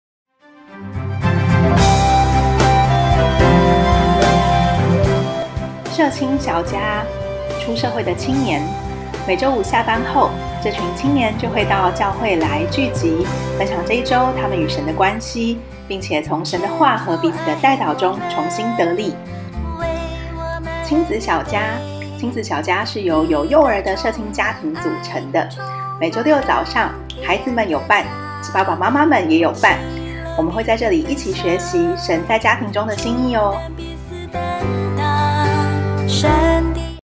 教會小家介紹---『社青介紹』『親子小家』
『社青小家』
專屬社會青年的小家，由完成學業進入職場、但還未婚的年輕人，在教會裡都歸屬於社會青年團契〈簡稱社青〉，但也有些還在唸書的研究生唷！🤓
歡迎對基督信仰有興趣的你來參加！

『親子小家』
每周六早上，會看到一群推著娃娃車的家長們，陸陸續續來到教會參與親子小家，教會就是我們屬靈的家。
歡迎家裡有小小孩們的家庭參加，讓我們在家庭、親子、信仰與教會生活等各個層面，交流分享，彼此代禱。
教會在親子小家時間，有同工協助小朋友的照顧，讓爸媽可以安心不被打擾，好好聚會唷~!
是不是很大心呢 🥰😍🥰😍

社青小家    每週五晚上 19：30～21：00
親子小家　週六上午 10：00～11：30
羅馬書12:2
不要效法這個世界，只要心意更新而變化，叫你們察驗何為神的善良、純全、可喜悅的旨意。

馬太福音19:14
耶 穌 說 ： 讓 小 孩 子 到 我 這 裡 來 ， 不 要 禁 止 他 們 ； 因 為 在 天 國 的 ， 正 是 這 樣 的 人 。